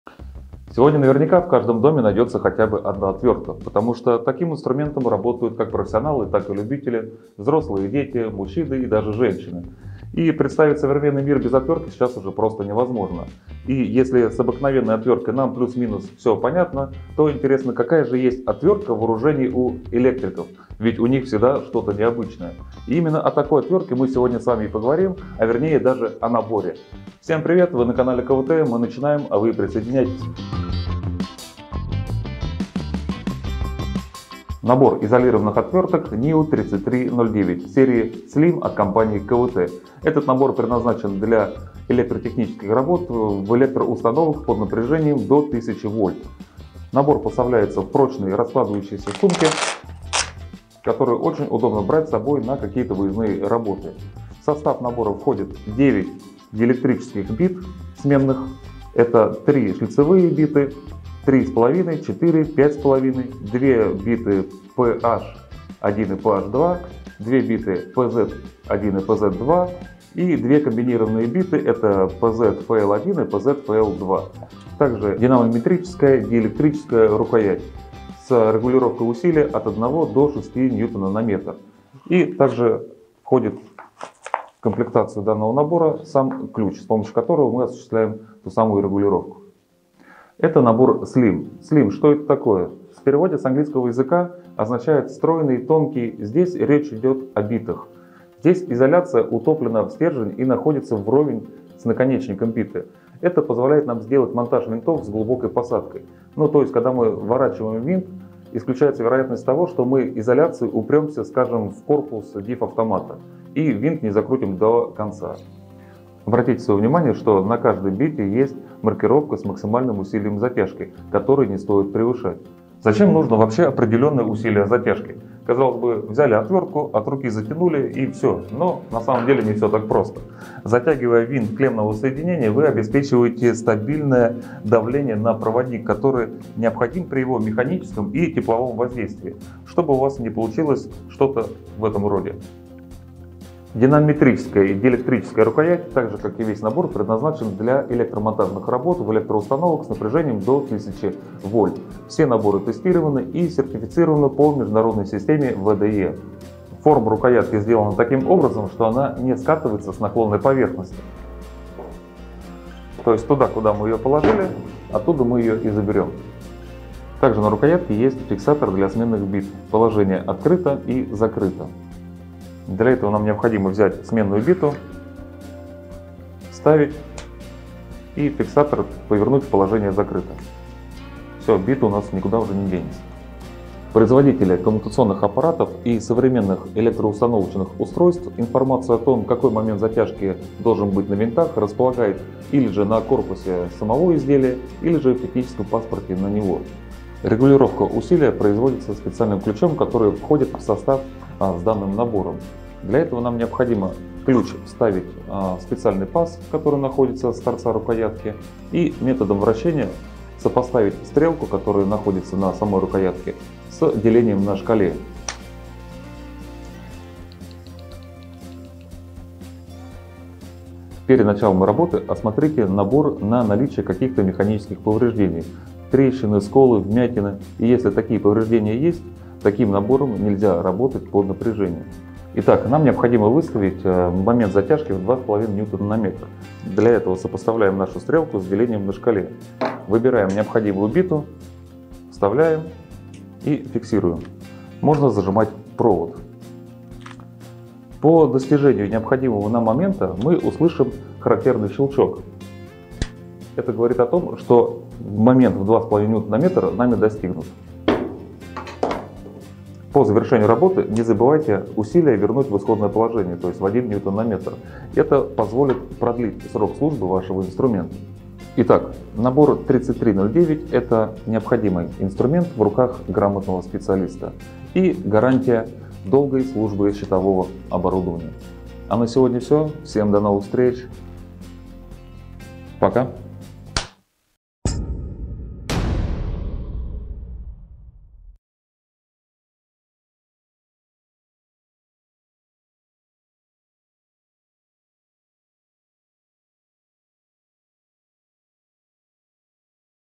Обзор набора НИО-3309 (КВТ): диэлектрическая динамометрическая отвертка VDE с 9-ю битами СЛИМ
НИО-3309
— Состав набора:
    • диэлектрическая бита: СЛИМ шлиц 3.5 х 70; max 0.6 Нм
    • диэлектрическая бита: СЛИМ шлиц 4.0 х 70; max 2.5 Нм
    • диэлектрическая бита: СЛИМ шлиц 5.5 х 70; max 5.5 Нм
    • диэлектрическая бита: СЛИМ PH 1 х 70; max 3.8 Нм
    • диэлектрическая бита: СЛИМ PH 2 х 70; max 5.5 Нм
    • диэлектрическая бита: СЛИМ PZ 1 х 70; max 3.8 Нм
    • диэлектрическая бита: СЛИМ PZ 2 х 70; max 5.5 Нм
    • диэлектрическая бита: СЛИМ PZ/FL1х70; max 3.8 Нм
    • диэлектрическая бита: СЛИМ PZ/FL2х70; max 5.5 Нм
    • диэлектрическая динамометрическая рукоятка
    • ключ для настройки усилия
— Для работы под напряжением до 1000 В
— Регулируемое усилие крутящего момента: 1–6 Нм
— Утопленная в лезвие отвертки СЛИМ изоляция делает возможным
     монтаж винтов с глубокой посадкой
— Материал стержня биты: хром-молибденовая сталь
— Намагниченные наконечники
— Материал изоляции стержня: полипропилен
— Упаковка: прочная раскладывающаяся сумка
— Вес набора: 480 г